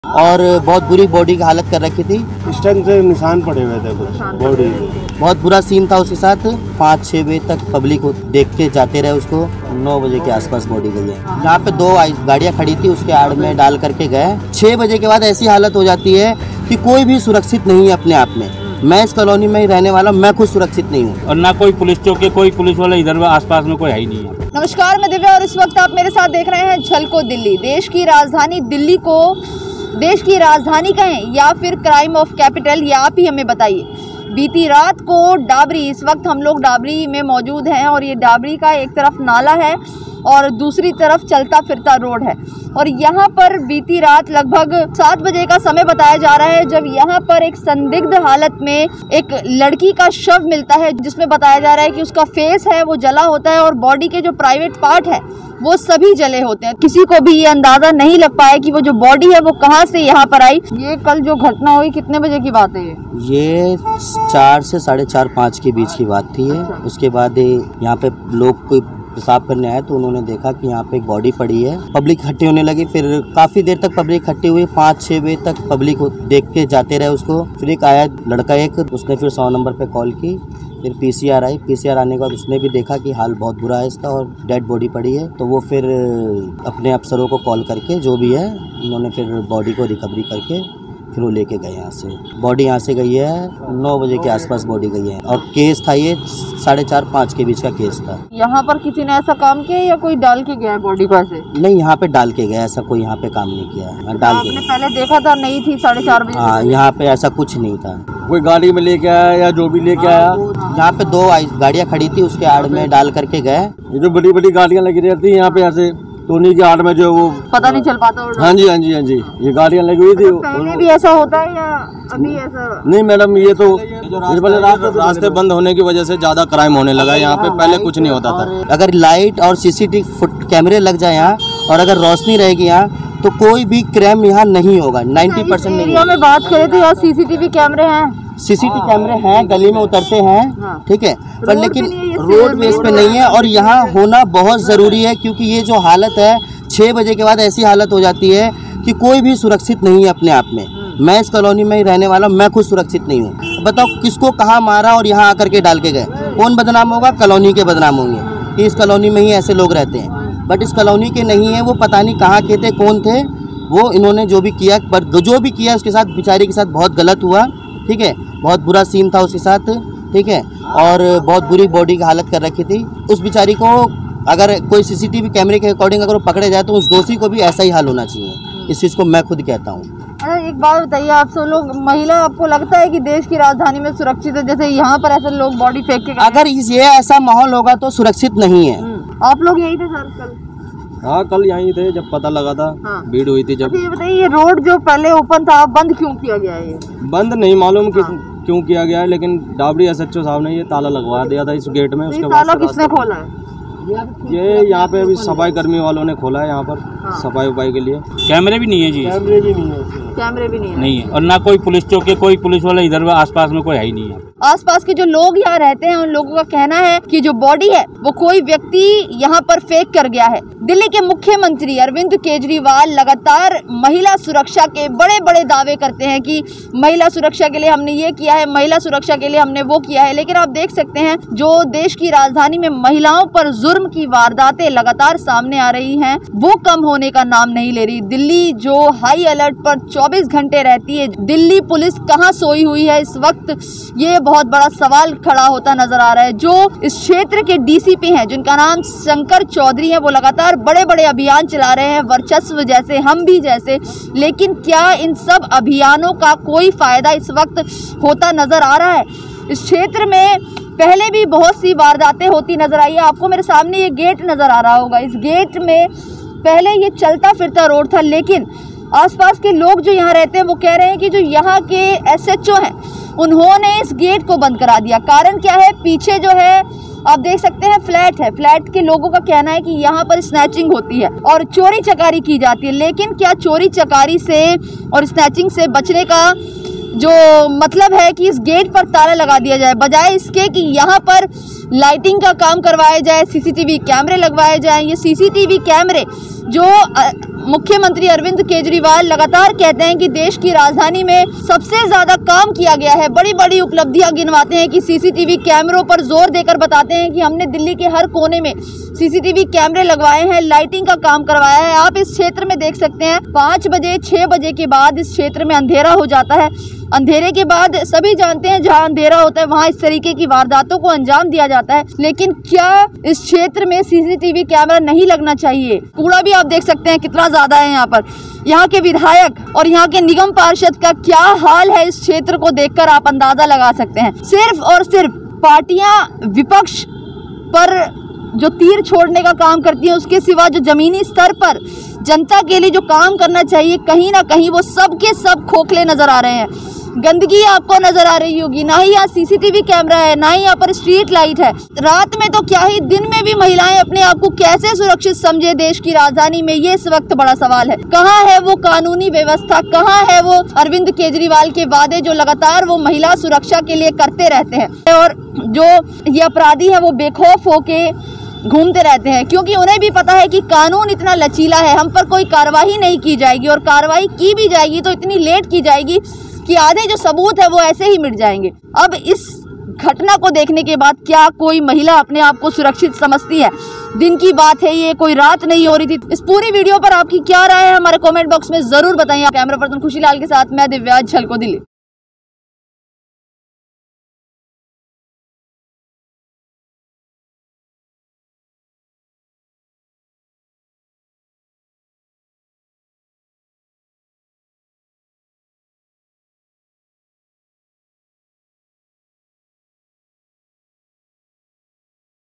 दिल्ली में रूह कपाती तस्वीरें महिला को जलाकर फेंका, कमजोर दिल वाले ना देखें ~ Delhi News
(JhalkoDelhi) झलको दिल्ली की टीम डाबरी (DAbri) क्षेत्र के मधु विहार (Madhu Vihar) बस स्टैंड के पास पहुंची थी जहाँ पर एक महिला की लाश बरामद हुई थी जिससे की पूरे इलाके में सनसनी छाई हुई है व पूरा मामला क्या था यह आपको वीडियो में देखने को मिलेगा इसीलिए हमारा आपसे अनुरोध है की आप पूरा वीडियो अंत तक देखें और ज़्यादा से लोगों तक शेयर करे ताकि अन्य लोग भी जागरूक हो और यदि आपको हमारी वीडियो अच्छी लगे तो चैनल का सब्सक्राइब करे !

Produced By: Jhalko Delhi
Edited By:  Amit
Reported By : Divya
Cameraman : Khushilal